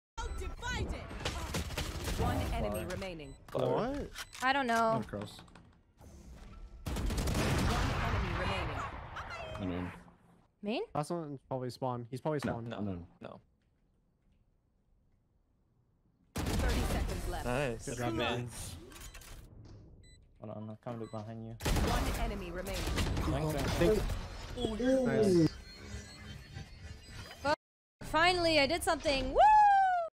Kyedae impressing Sen Tenz in the Immortal lobby
Send your clip

If you liked it please follow the streamer